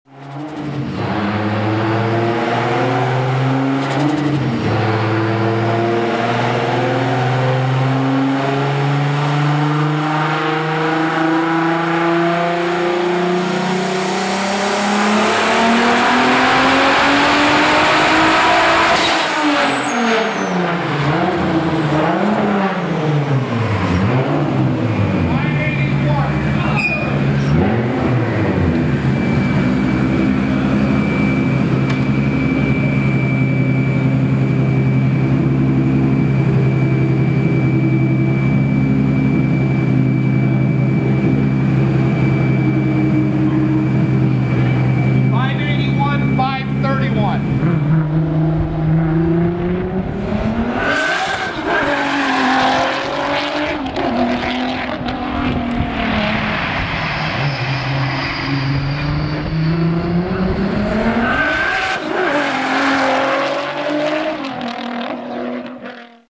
Golf R32 Turbo 742 BHP
The turbo system is based on the original design of the Stage 1 and Stage 2 turbo systems featuring a similar piping layout and OEM like look. At the core of the system is a Custom Turbonetics T04 based custom turbocharger, EIP custom front mount intercooler utilizing the Spearco bar and plate core along with EIP's upgraded intake and charge piping. Internally the bottom end of the engine has been fitted with EIP forged pistons and chromemoly rods that are mated to an EIP High Flow Cylinder Head and EIP custom head studs. The fuel system is upgraded with EIP custom direct fit 1000cc fuel injectors, 1:1 fpr, upgrades fuel lines and fuel pump. Managing these components is the Electromotive TEC3 Stand Alone Engine Management System. An EIP Transmount, custom Race series clutch, pressure plate and flywheel manage the OEM drive line. The AWD system incorporates a custom Haldex RWD controller that enables 20/80, 50/50 and other torque splits between the front and rear drive.